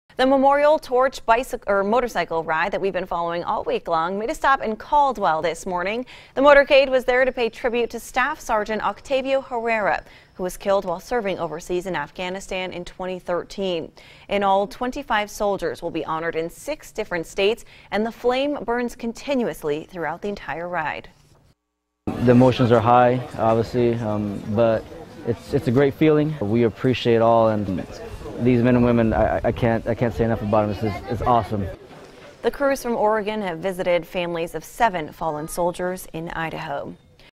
Memorial Torch Motorcycle Ride
The motorcade was there to pay tribute to Staff Sgt. Octavio Herrera, who was killed while serving overseas in Afghanistan in 2013.